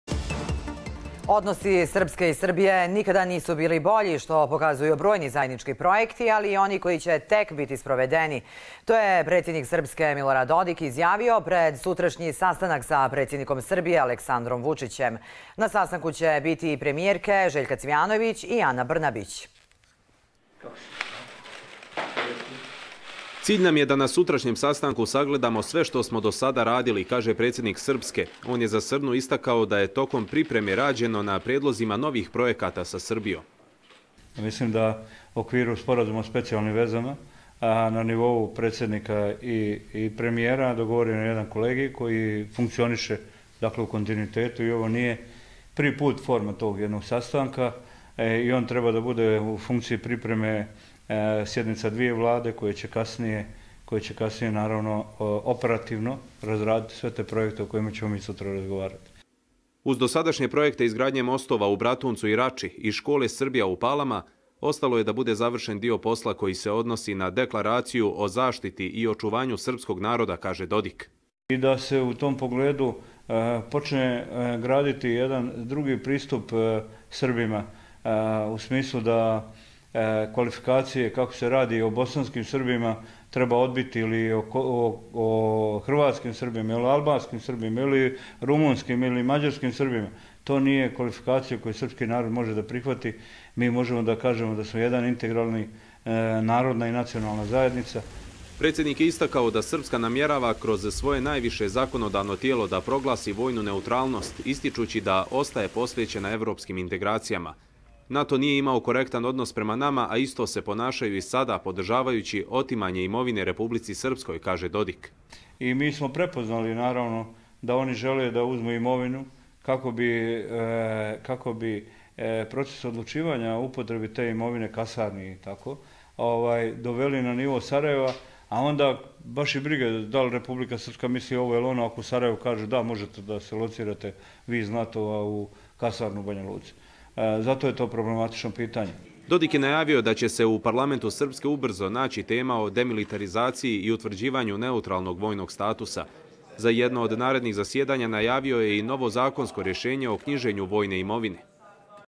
Dodik: Odnosi Srpske i Srbije nikad bolji, završavamo deklaraciju
Odnosi Srpske i Srbije nikad nisu bili bolji, što pokazuju brojni zajednički projekti, ali i oni koji će tek biti sprovedeni, izjavio je predsjednik Srpske Milorad Dodik, pred sutrašnji sastanak sa predsjednikom Srbije Aleksandrom Vučićem. Na sastanku će biti i premijerke Željka Cvijanović i Ana Brnabić.
goo.gl/CXPZBy